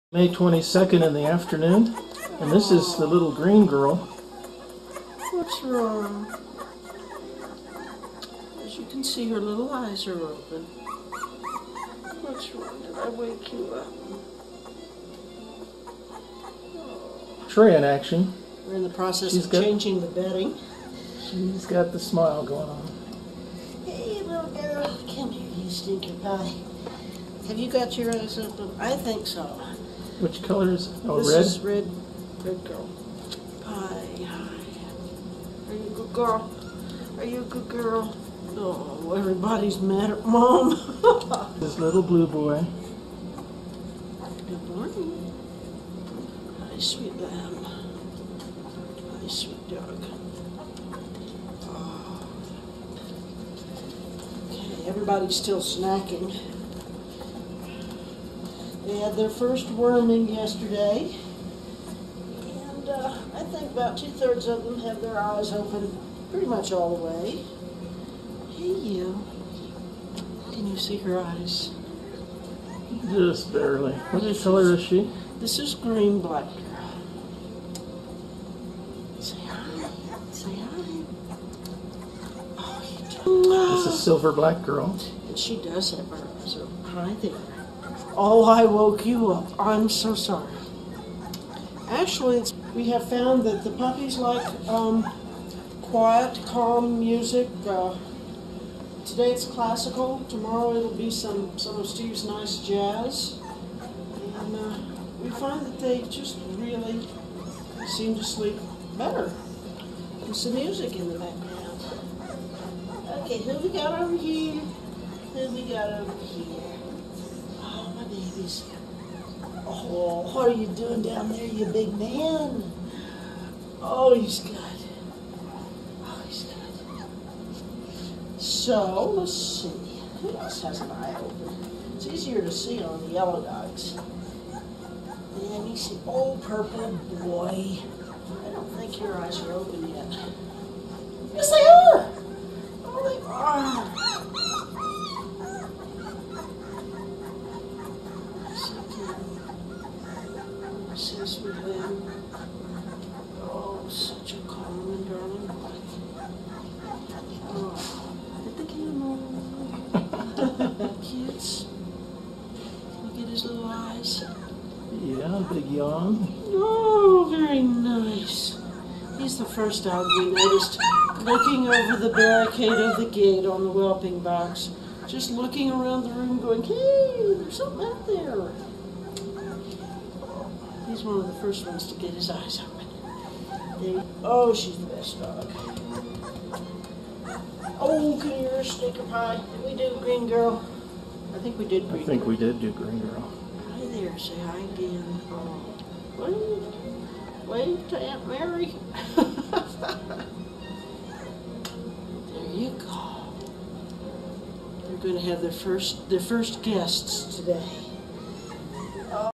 Sunday May 22, 2011 eyes opening
Every morning we get a new surprise who will have their eyes open!!  We'll do another quick video next weekend, at 3 weeks, when the pups start to eat semi-solid food!!